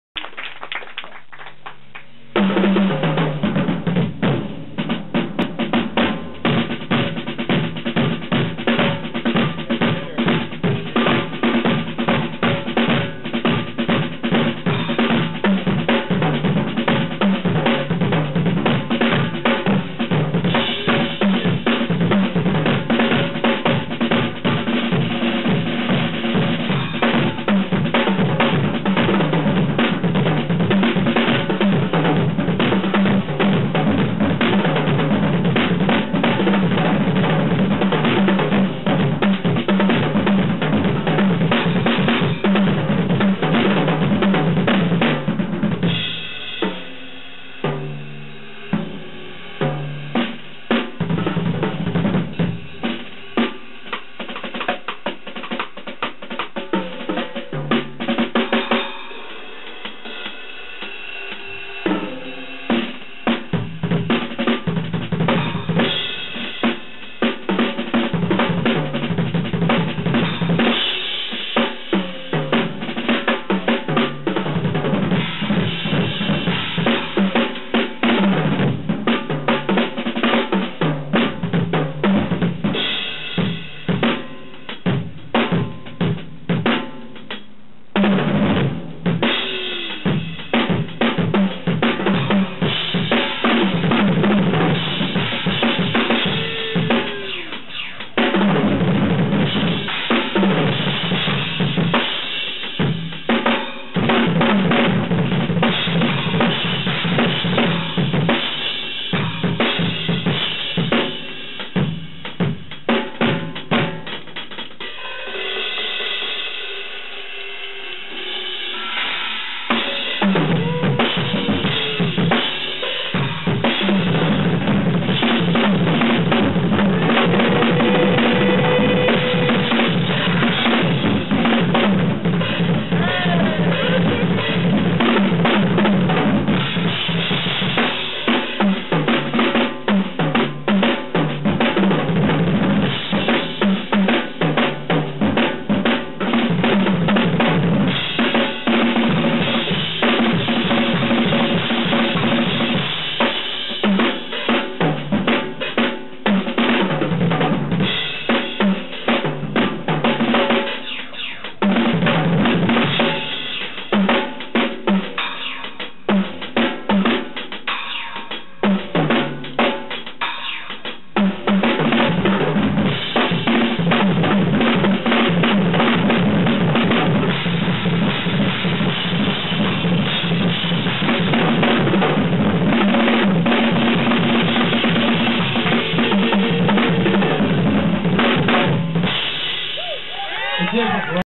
Shockwave Magazine: Guitar Center Drum Off 2011: Brian Williams
Brian Williams at Guitar Center's 2011 Drum Off in Towson, Maryland.